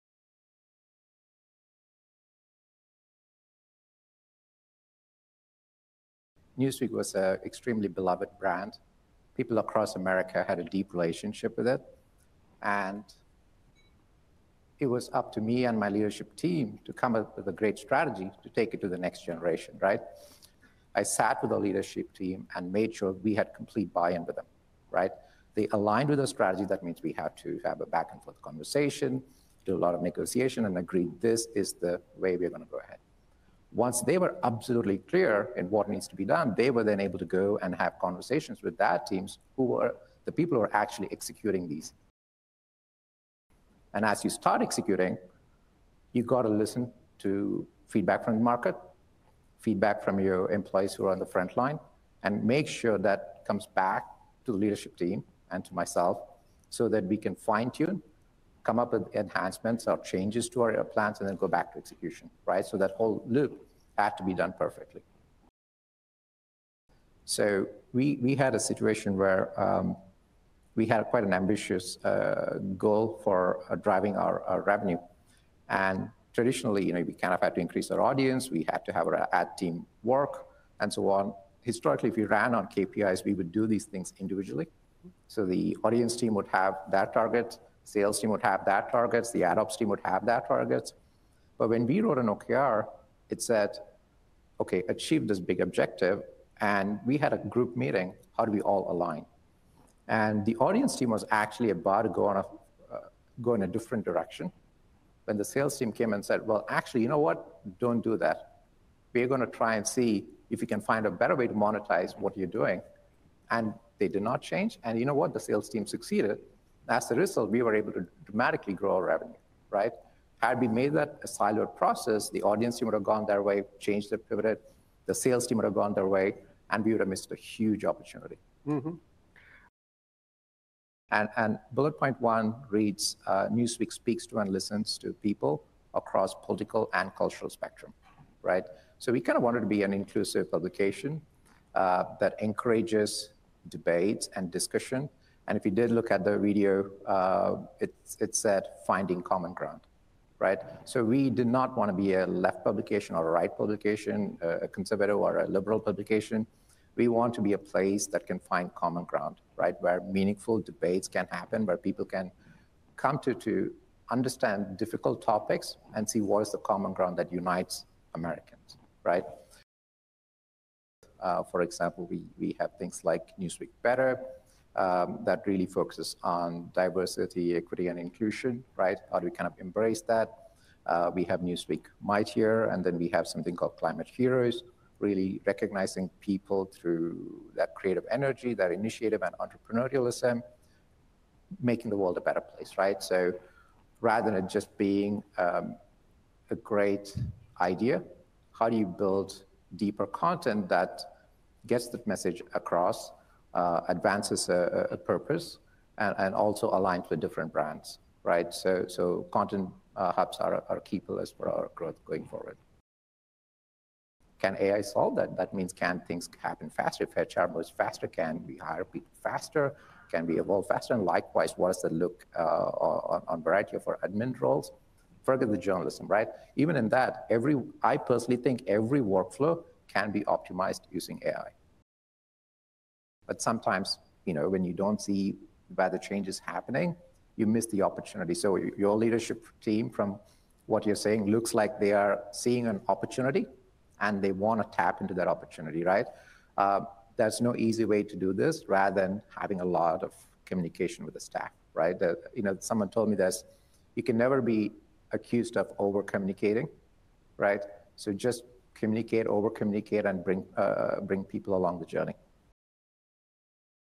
Global Communicators Conference
The Conference Board Global Communicators Conference. March 2023 Keynote interview with Dev Pragad.